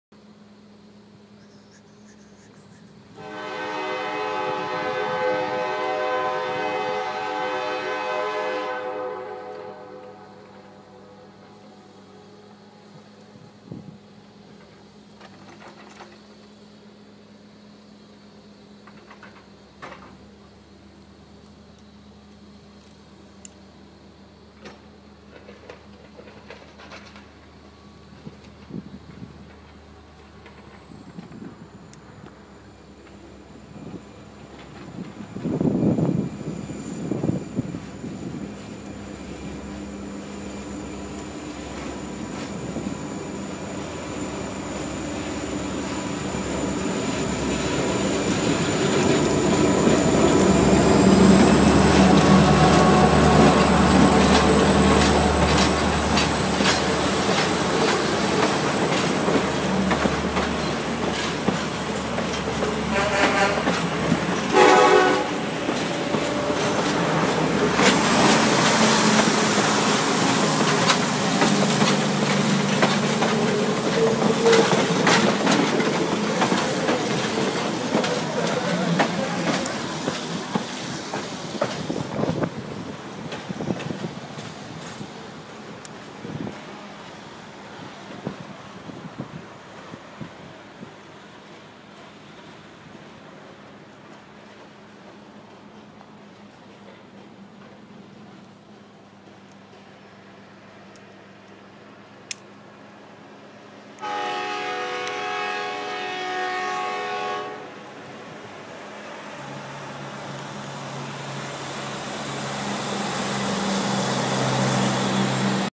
HD: The Conway Scenic; A Mini-Series. Part9
Date: September 26, 2012; 1249 Taken with an iPhone 4S

A timeless shot that anybody who's railfanned the Mountain Division has taken; a northbound train coming through the Gateway. That includes my father, who instilled my passion for railroads in me. He has hundreds of photo slides and stories of railfanning the line back in the 1970s when mile long freight trains still plied these rails. I've slowly been trying to recreate his scenes in photo and in video, and this was an absolute must. Note the elusive mountain climbing Wendell up on the rock cut. A very hard species to capture on film, though often heard, this example shows their rock climbing habits. /end joke.

Oh, and the clanking noises heard in the video (besides the train!) was a construction crew repairing retaining walls and cleaning slide zones a hundred yards or so down the mountain.